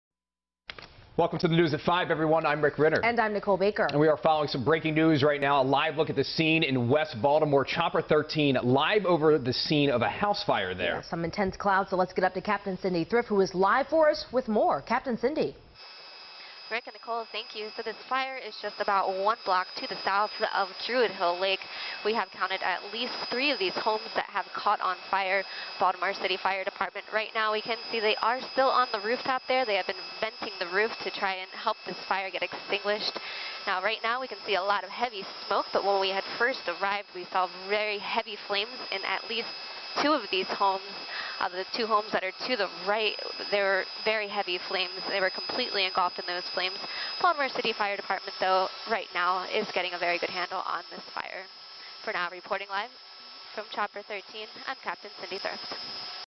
Firefighters On Scene Of Rowhome Fire In West Baltimore
Baltimore fire officials are on the scene of a second-alarm fire in West Baltimore.